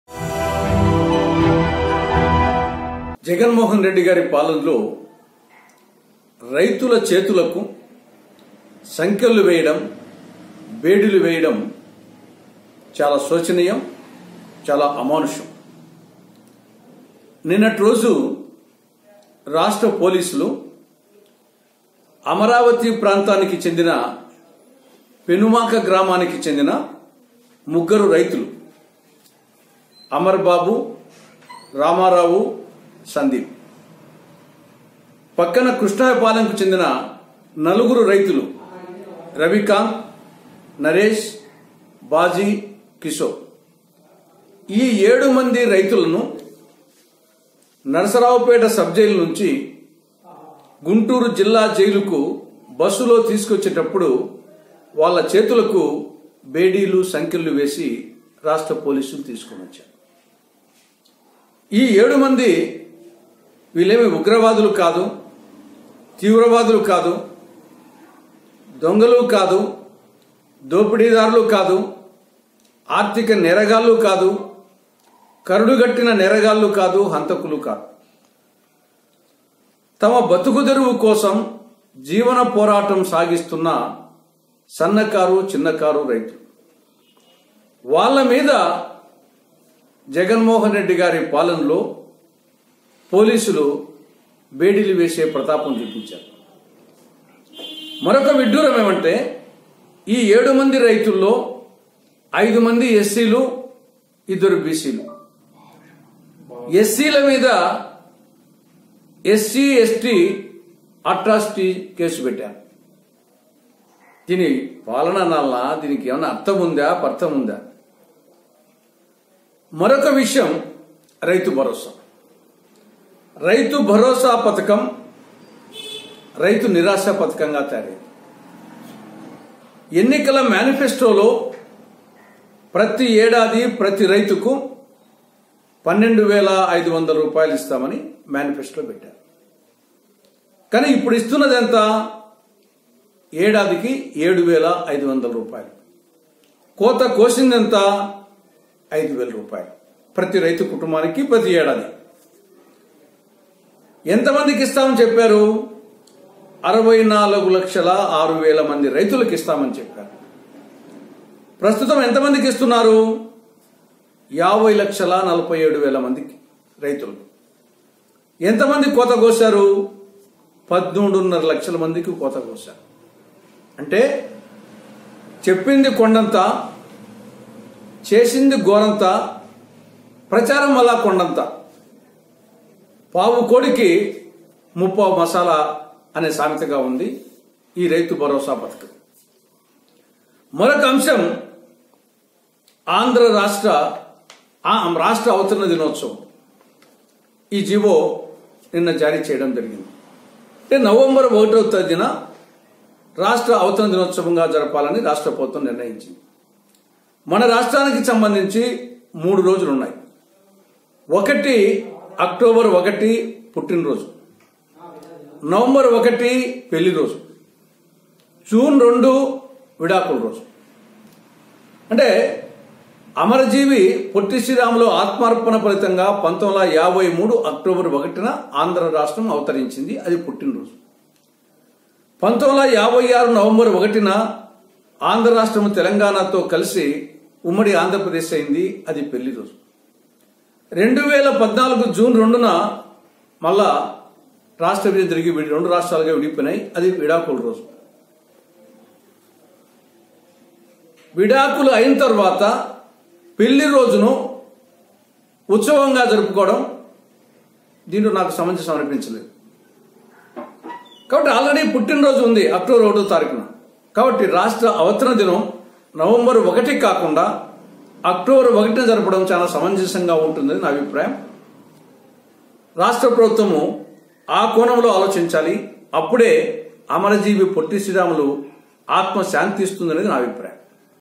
రైతు భరోసా పథకం..రైతు నిరాశ పథకంగా తయారైంది | Tulasi Reddy Criticising Comments | Tolivelugu TV
రైతు భరోసా పథకం..రైతు నిరాశ పథకంగా తయారైంది